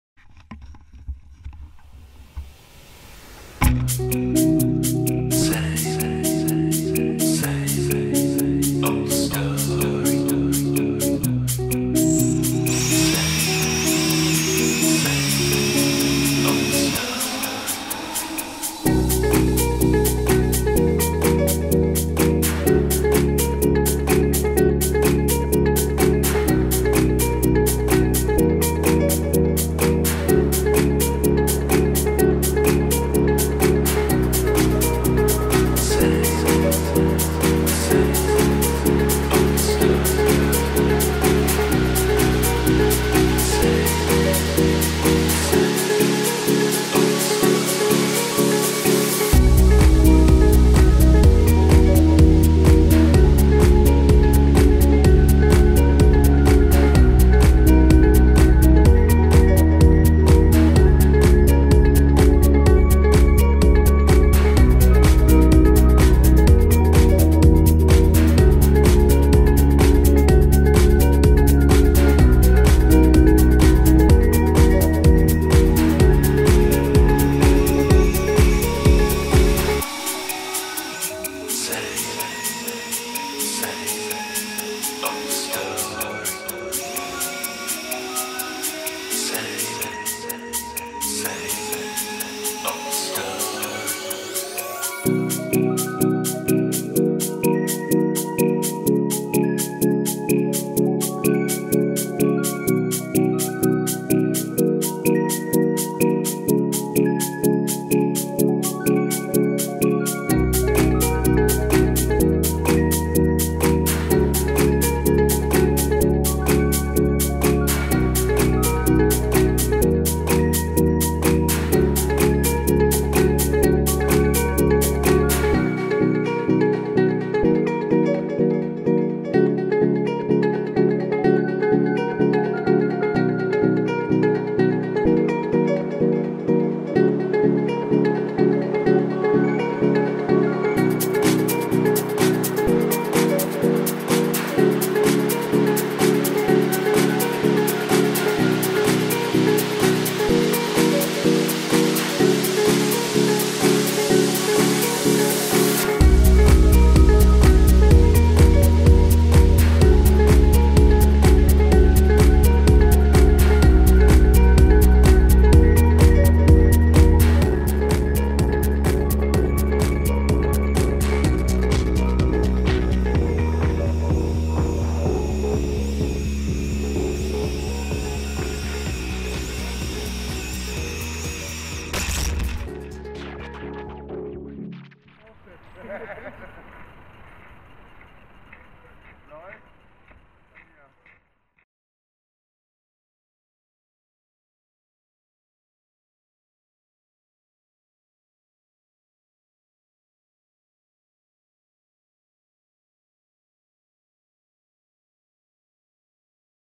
Gravel Plant FpV ( Uncut one take)
Little FpV Session at the gravel plant...
Setup:

Frame Lisam 210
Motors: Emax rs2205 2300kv
Esc: Littlebee30a
Fc: Kiss
Props: Gemfan5050x3

Hd cam xiaomi yi@ 1280x720